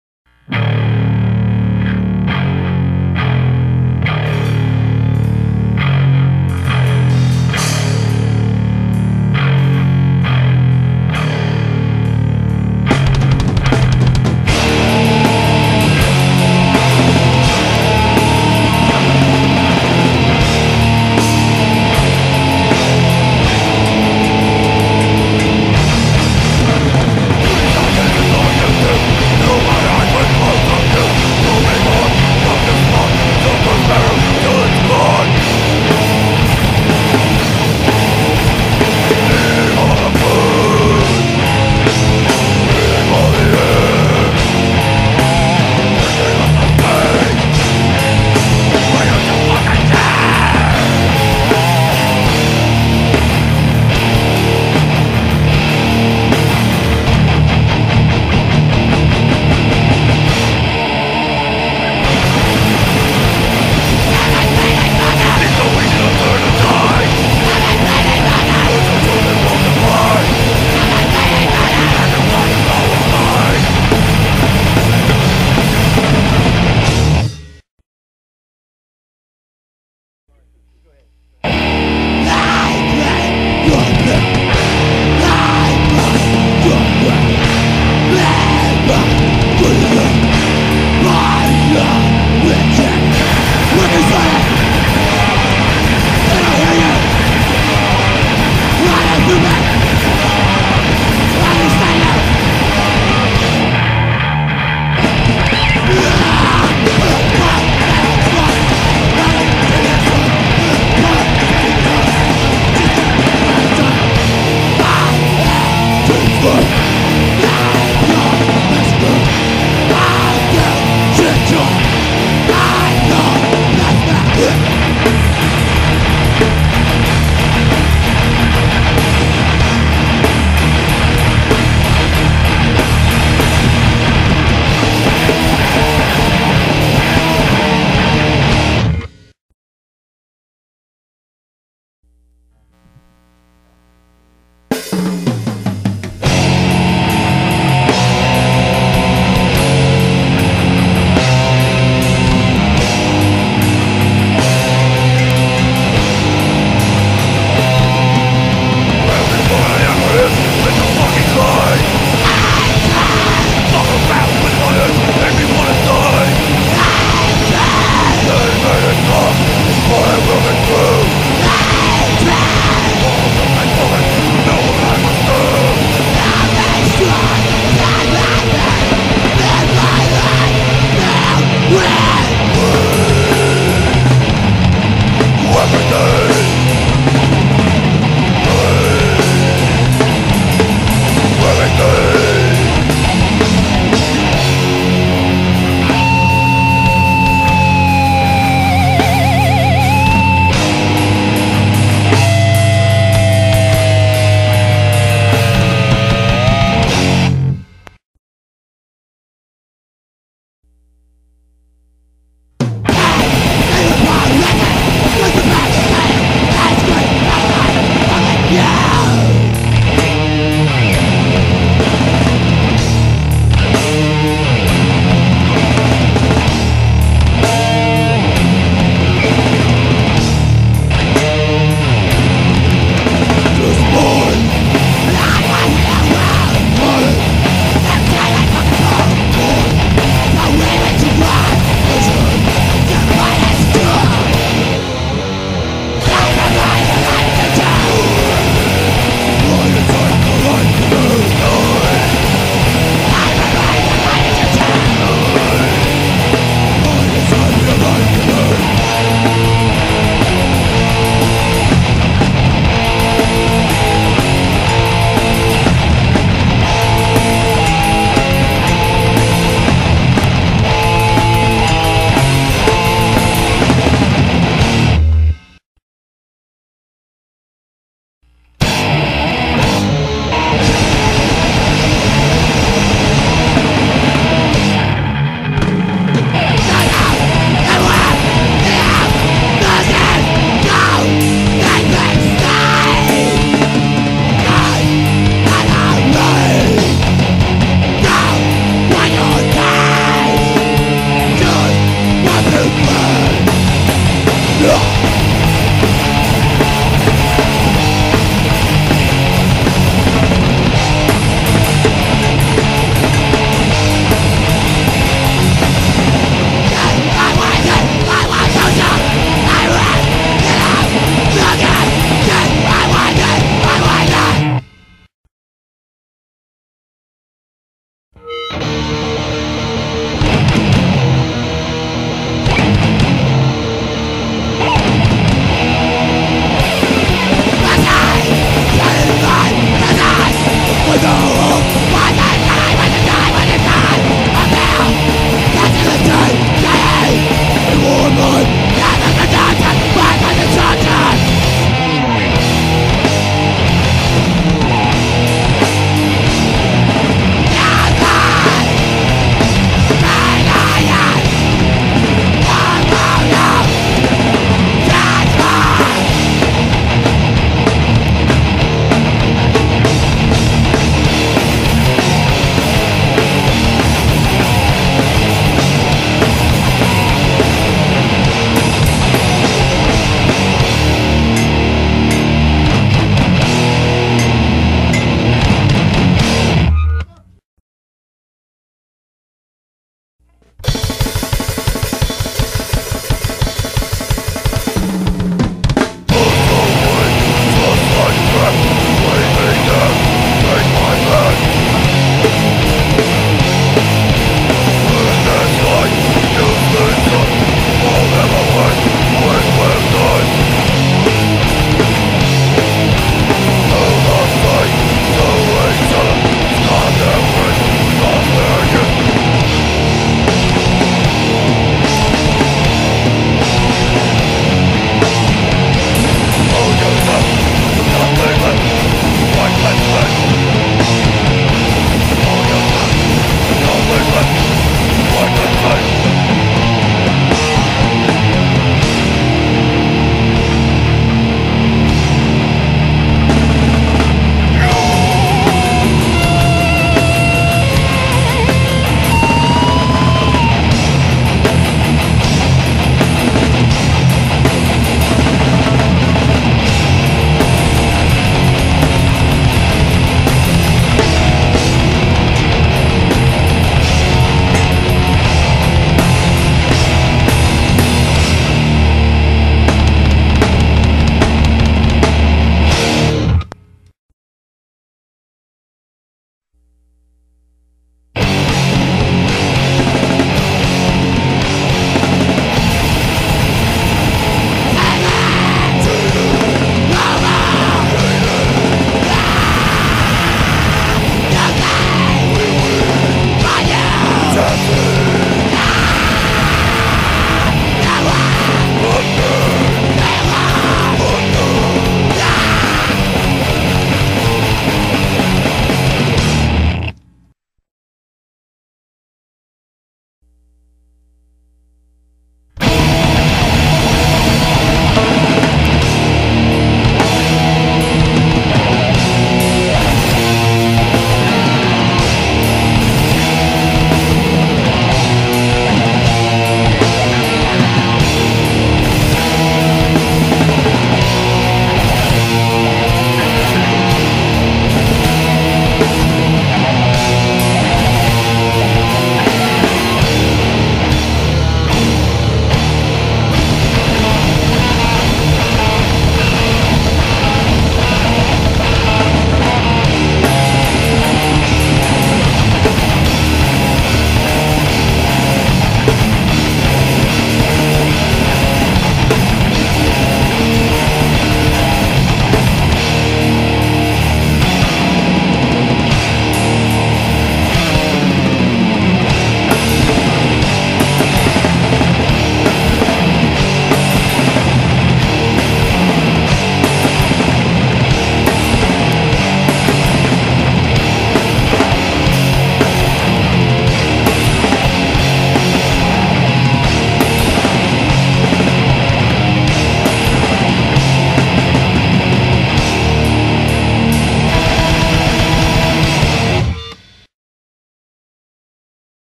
League of Struggle hardcore thrash
League of Struggle 9 songs from 2004ish a few pictures at the end
L.O.S. Haltom City Hardcore